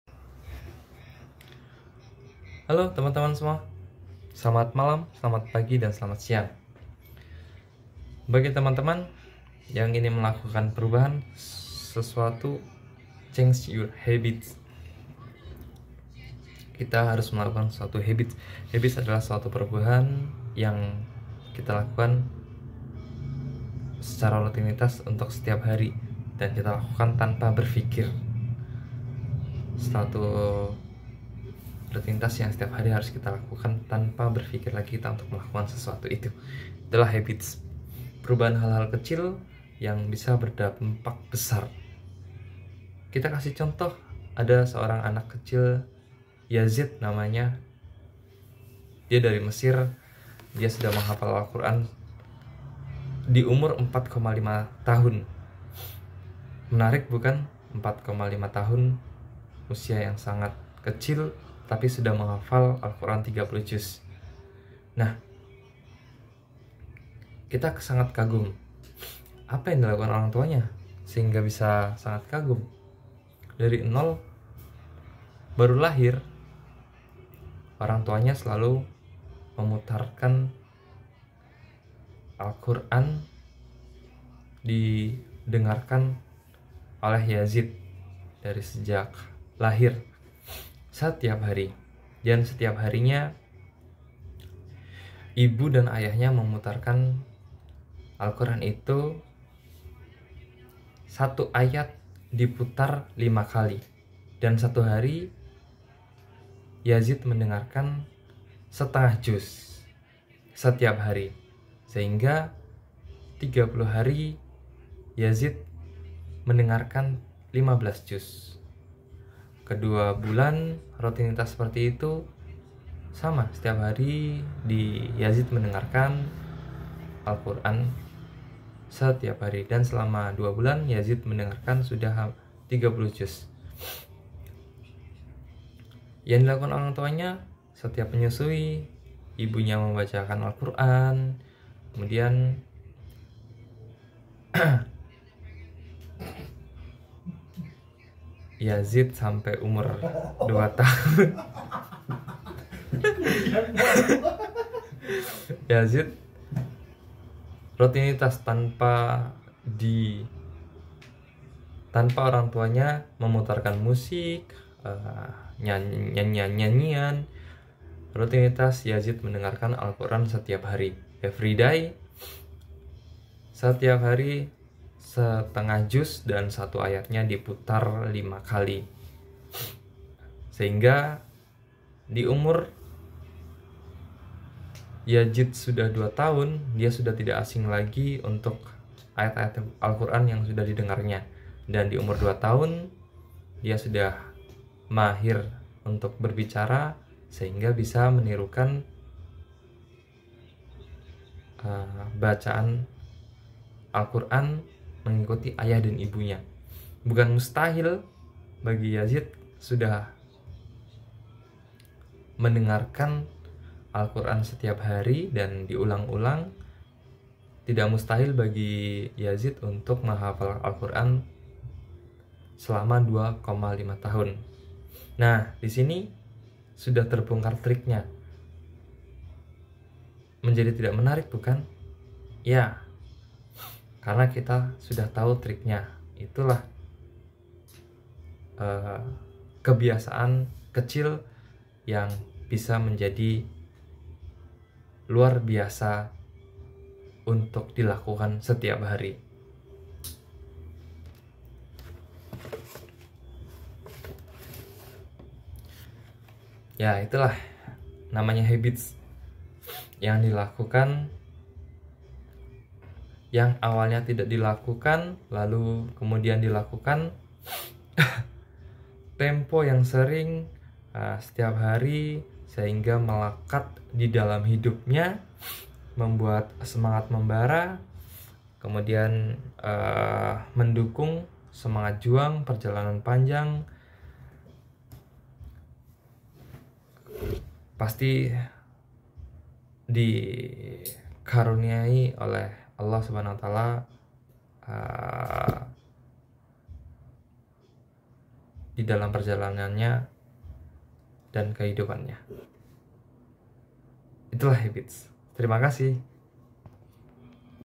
Habits !!! Hal kecil menjadi besar
habits segala sesuatu yang awalnya tidak lakukan, lalu di lakukan dalam waktu yang sering dan setiap hari, membuat semangat membara.
menjadikan sesuatu yang bukan apa apa menjadi sesuatu yang menakjubkan dan luar biasa.